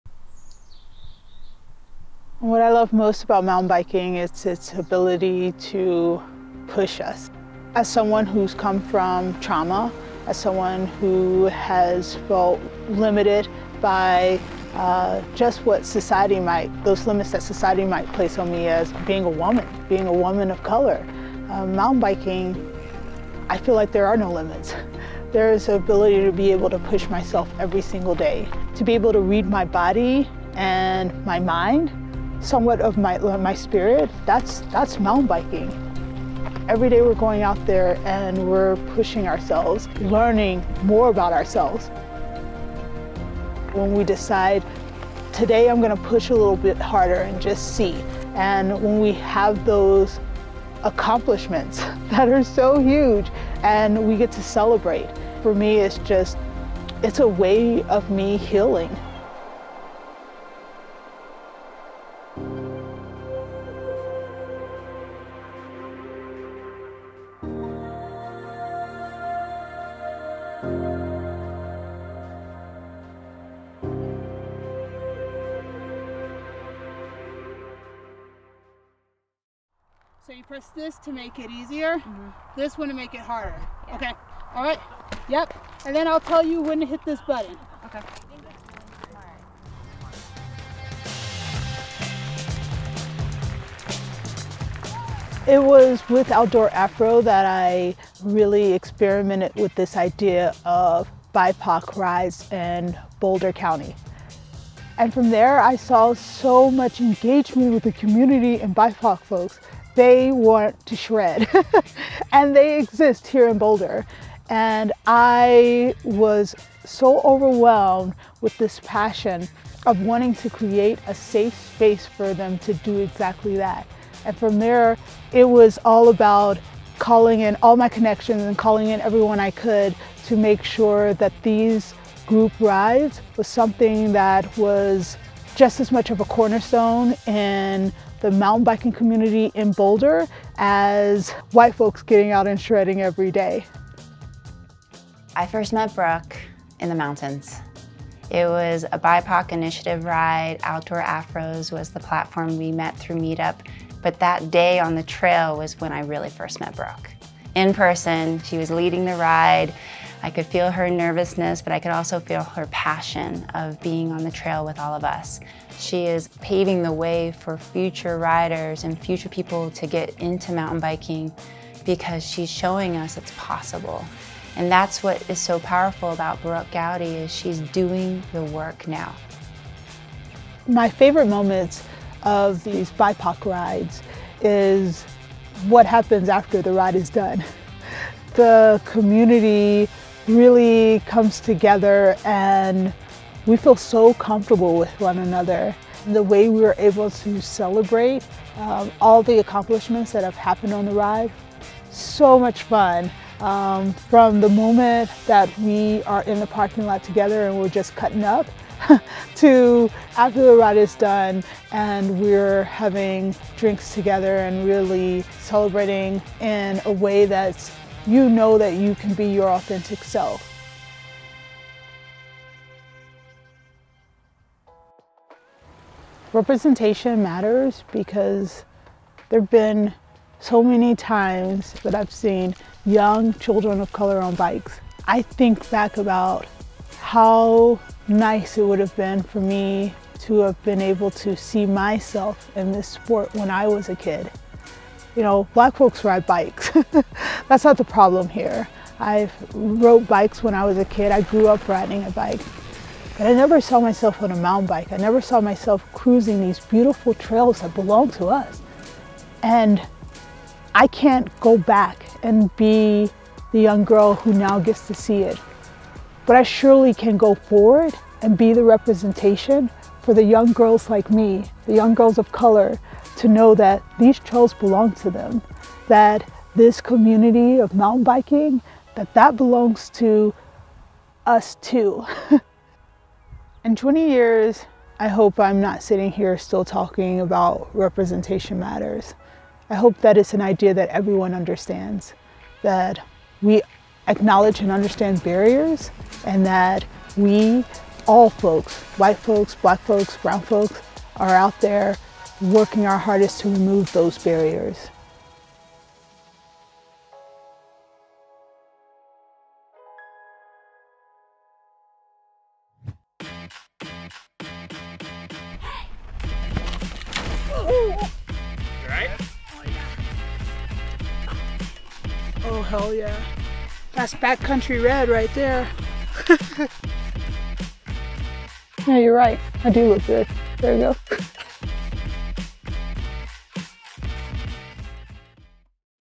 A Space For All - Rider Brooke Goudy's Mission for Representation
Brooke Goudy is on a mission to not have to talk about how representation matters. Getting all people to "acknowledge and understand barriers, and that we, all folks, white folks, black folks, brown folks, are out there working our hardest to remove those barriers."
Resilience: It is the key to success on and off the trail. It's the ability to fall and get up again, to recover from your worst day and hold excitement for tomorrow. Off the bike, we engage in conflict and struggle with those around us, most often with ourselves. We build resilience on the trail and carry it into our daily lives. It is what gives us strength.

Strength: Right foot, left foot – the biking rhythm keeps us upright during the hardest part of the ride. A mantra that carries us through to the end. We grow strong, learn to lean into the discomfort, and stand up to the challenge. It is not easy to be the "only one," to challenge the status quo, to make room for others, but we do it. We persevere and tap into our grit.

Grit: We set goals and follow through with early morning rides and late-night meetings. We advocate for access to trail systems and volunteer to keep them in good repair. Our passion drives our obsession. We are not afraid to speak our truth and reckon with the impact of our choices. Resilience, Strength, and Grit: this is MTB. These trails belong to everyone. We must work to make "a space for all."
-by Brooke Goudy

Film by Drew Bennett Productions